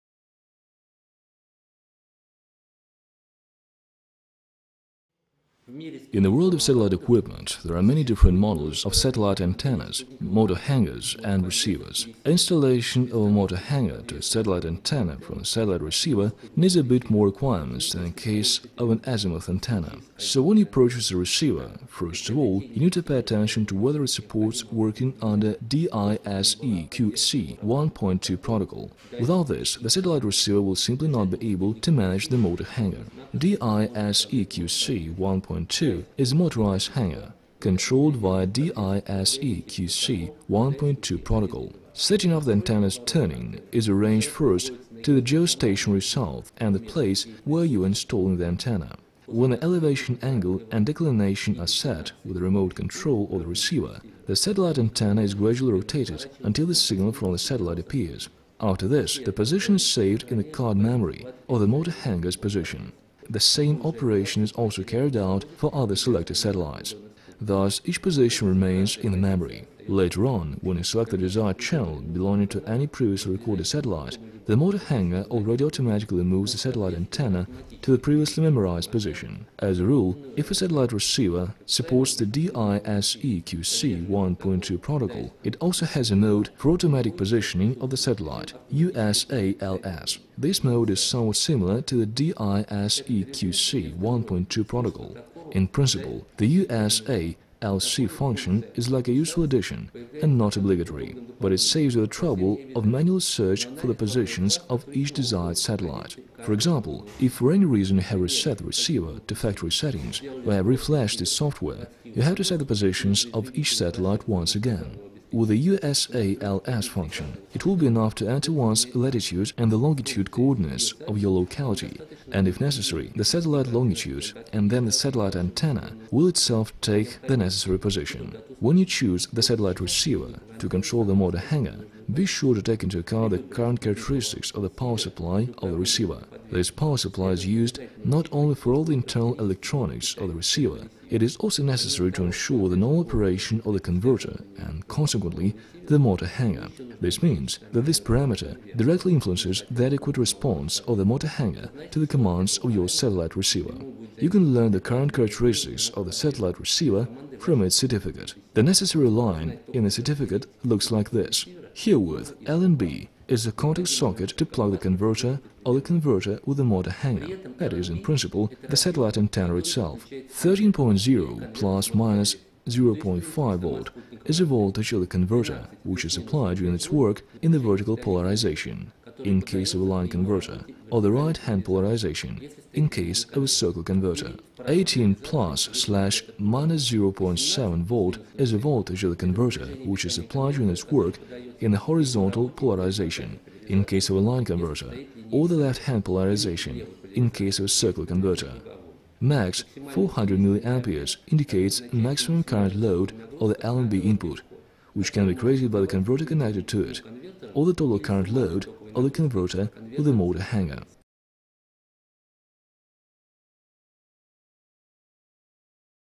Choice of a receiver for motorized satellite ...
Choice of a receiver for motorized satellite antenna        In the world of satellite equipment, there are many different models of satellite antennas, motohangers and receivers. Installation of a motohanger on a satellite antenna from the satellite receiver itself needs a bit more requirements than in case of an azimuth antenna.       So, when you purchase a receiver, first of all you need to pay attention to whether it supports working under DiSEqC 1.2 protocol. Without this the satellite receiver will simply not be able to manage the motohanger.       DiSEqC 1.2 is a motorized hanger, controlled via DiSEqC 1.2 protocol. Setting up of the antenna's turning is arranged first to the geostationary south of the place where you are installing the antenna. When the elevation angle and declination are set with the remote control of the receiver, the satellite antenna is gradually rotated until the signal from the satellite appears. After this the position is saved in the card memory of the motohanger's positioning. The same operation is also carried out for other selected satellites.       Thus, each position remains in the memory.       Later on, when you select the desired channel belonging to any previously recorded satellite, the motohanger already automatically moves the satellite antenna to the previously memorized position. As a rule, if a satellite receiver supports the DiSEqC 1.2 protocol, it also has a mode of automatic positioning to the satellite - USALS.       This mode is somewhat similar to the DiSEqC 1.2 protocol. In principle, the USALS function is like a useful addition, and not obligatory. But it saves you the trouble of manual search for the positions of each desired satellite. For example, if for any reason, you have reset the receiver to factory settings, or have re-flashed its software, you'll have to set the positions of each satellite once again.       With the USALS function it will be enough to enter once the latitude and the longitude coordinates of your locality, and, if necessary, the satellite longitude, and then the satellite antenna will itself take the necessary position.       When you choose a satellite receiver, to control the motohanger, be sure to take into account the current characteristics of the power supply of the receiver. This power supply is used not only for all the internal electronics of the receiver, it is also necessary to ensure the normal operation of the converter and, consequently, the motohanger. This ...